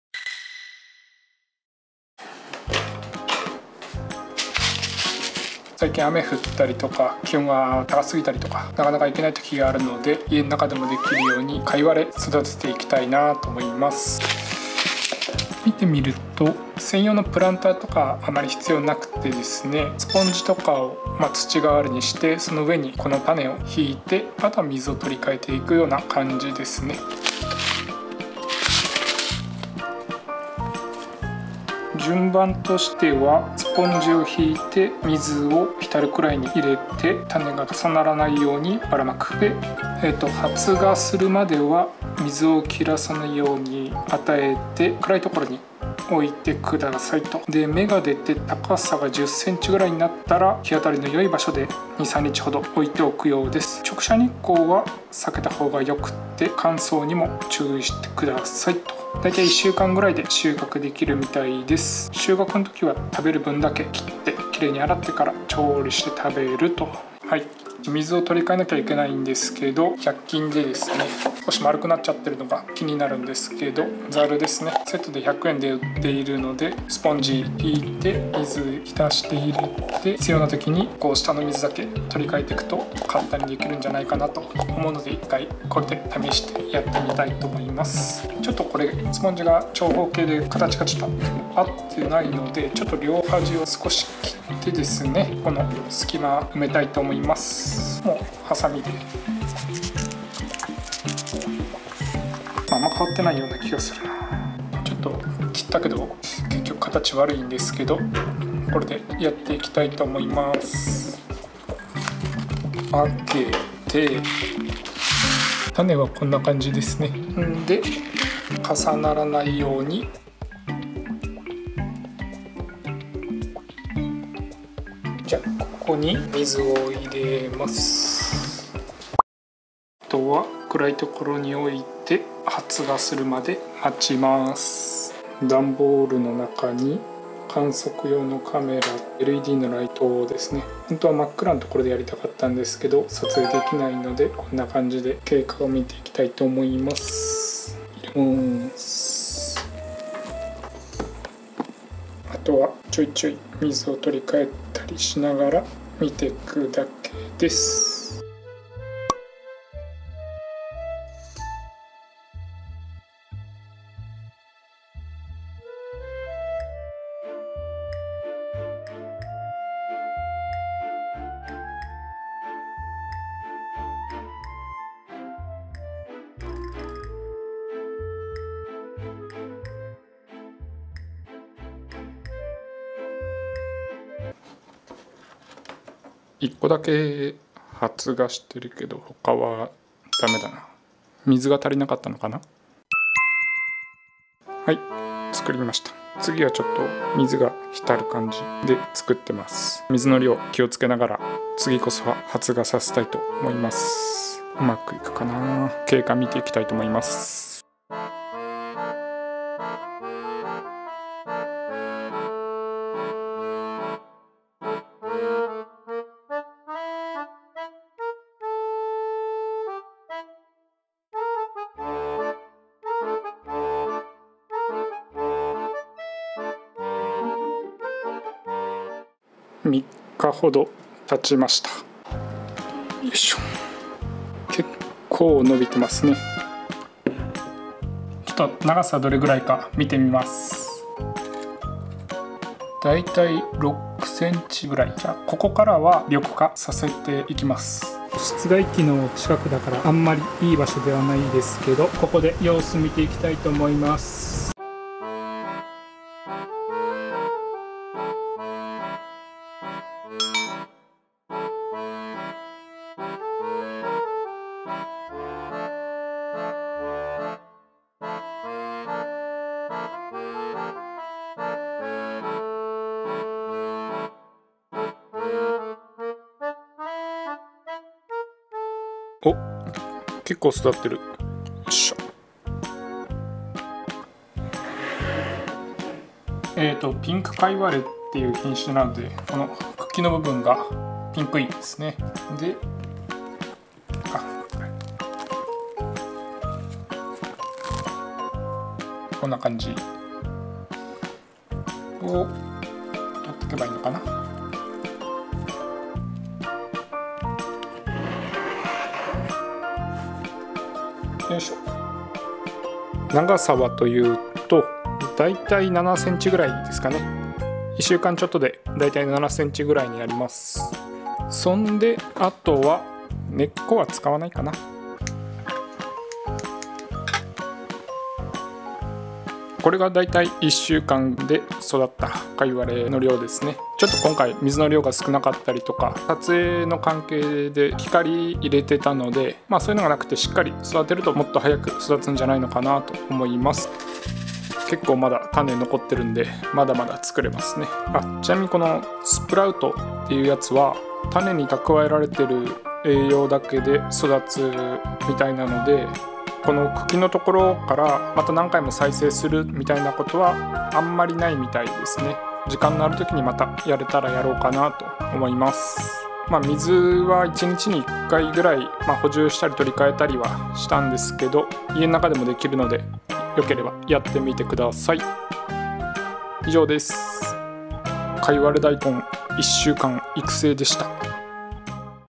【水耕】1週間で収穫！かいわれ大根を育ててみた
水耕をしたいのでまずはその足がかりとして一番楽なかいわれ大根を育ててみました。
知っていると思いますが家の中でも栽培可能なかいわれ大根を作りました。

種はスプラウト用に売られているものを使用しました。

使うのは↓の3つだけです。
・種
・ボウル
・スポンジ

水さえ切らさなければある程度はしっかり作れるので是非お試しくださいませ。

ちょっと一回目は水が少なすぎて失敗してしまいましたが再挑戦したらうまくいきました。
一回目に使った種も水をしっかりあげたら復活してかいわれ大根になったので。失敗したけどなんとか巻き返せてよかったです。

家庭菜園で畑を耕すのも良いですが、誰でも手が出せるということで今回は水耕を少し始めてみました！という動画です。